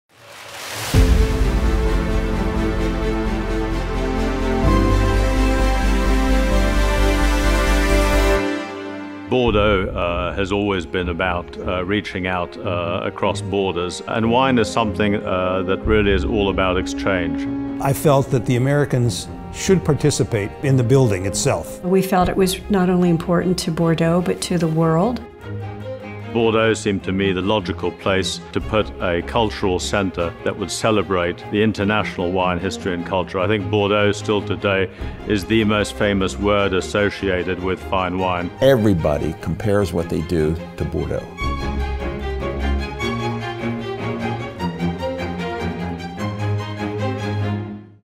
Bordeaux et sa Cité du Vin - FR
"Bordeaux a toujours eu un statut l'amenant au-delà des frontières, et le vin est par définition une question d’échanges." S.A.R. Prince Robert de Luxembourg

Son S.A.R. Prince Robert de Luxembourg (PDG Domaine Clarence Dillon), Denise Adams (propriétaire de Château Fonplégade à Saint-Emilion, ADAMVS en Napa Valley), Robert Parker (célèbre critique vin américain), et l'américain George P. Sape (Président des Amis du Vin de la Cité du Vin) nous parlent de la place qu'occupe Bordeaux et de leur implication dans le projet de création de la Cité du Vin de Bordeaux.

Retrouvez ces personnalités dans le documentaire "Eastbound Westbound - A winemaker's story from Bordeaux and California" prochainement diffusé sur une plateforme S.V.O.D. américaine.